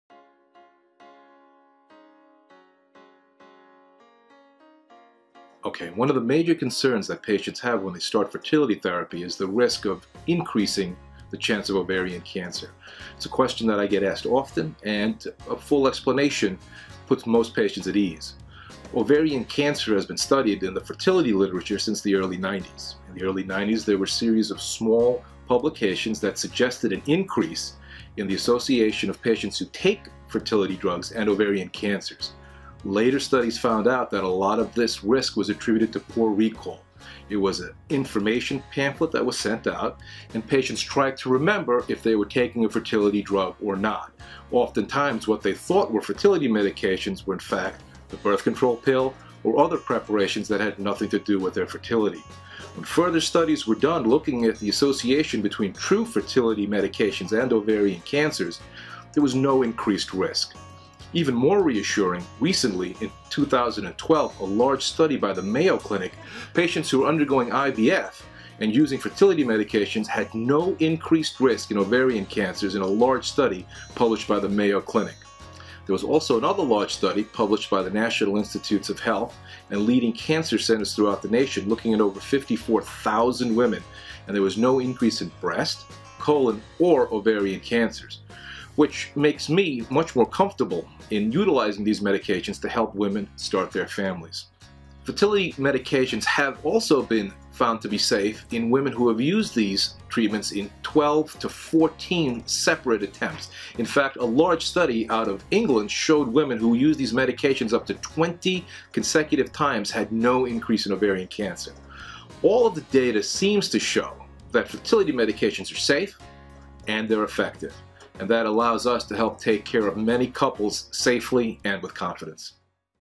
Dr. Tanmoy Mukherjee describes fertility medications and treatment options.
Dr. Tanmoy Mukherjee, Co-Director of RMA of New York, describes fertility medications and treatment options.  He includes an analysis of side-effects and health risks and benefits..